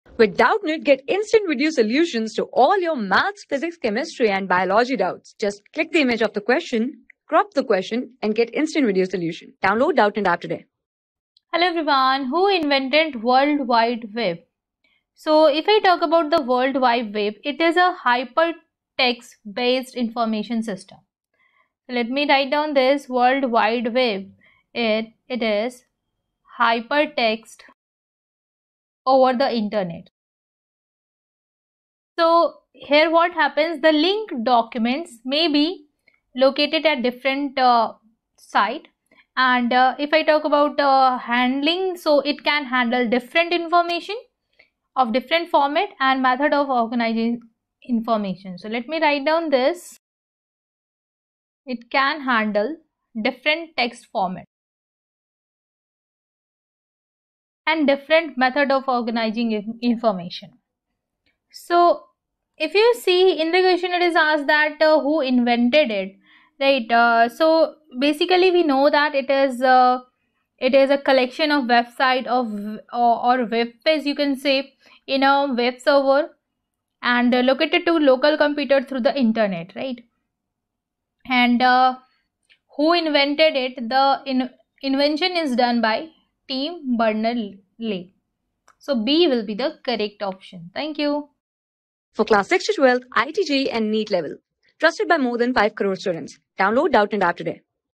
A non-uniform magnetic field vecB=B_(0)(1+y/d)(-hatk) is present in region of space in between y...
A non-uniform magnetic field vecB=B_(0)(1+y/d)(-hatk) is present in region of space in between y=0 & y=d.The lines are shown in the diagram.A particle of mass m and positive charge q is moving.Given an initial velocity vecv=v_(0)hati.Find the components of velocity of the particle when it leaves the field.
Class: 12
Subject: PHYSICS
Chapter: ELECTRODYNAMICS
Board:IIT JEE

👉 Have Any Query? Ask Us.